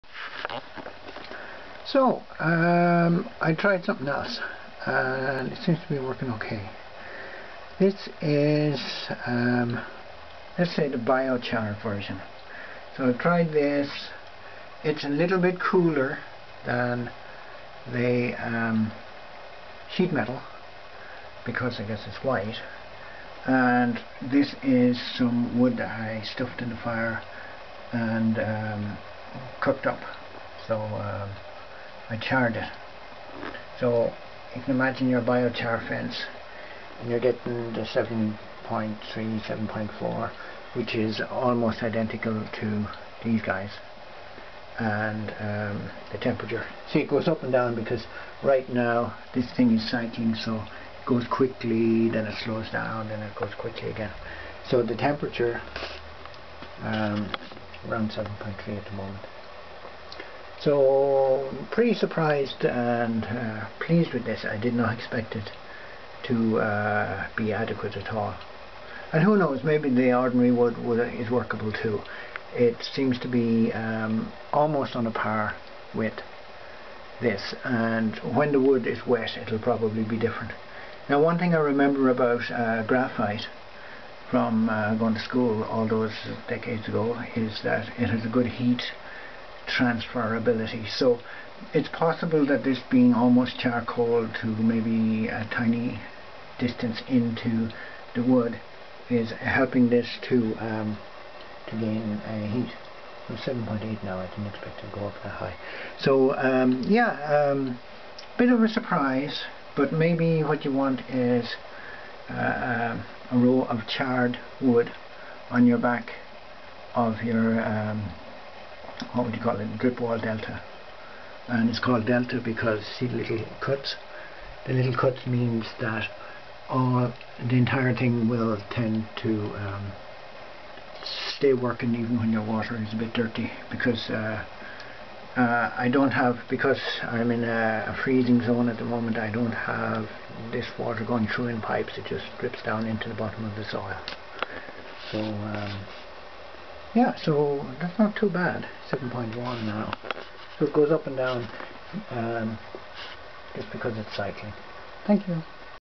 BioChar dripwall delta! Charred wood as a dripwall to warm the soil for seedlings.
Sheet metal works really well  for a solar warming drip wall, wood works ok, slate works well, how well will charred wood work?